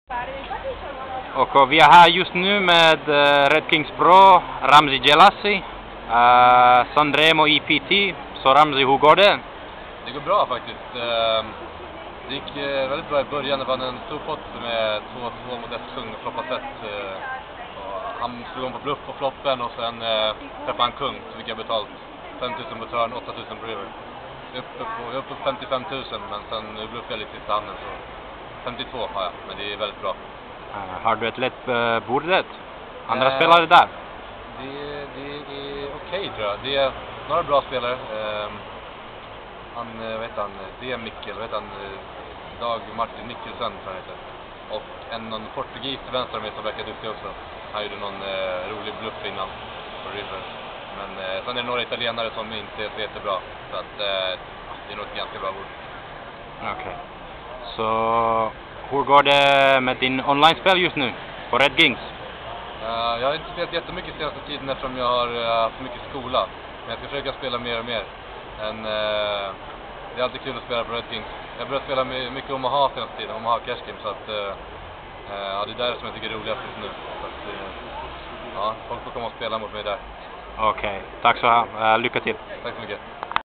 Ramzi Jelassi.AVI
Ramzi Jelassi speaking in swedish about his play at PokerStars EPT San Remo during the second break of day 1 a.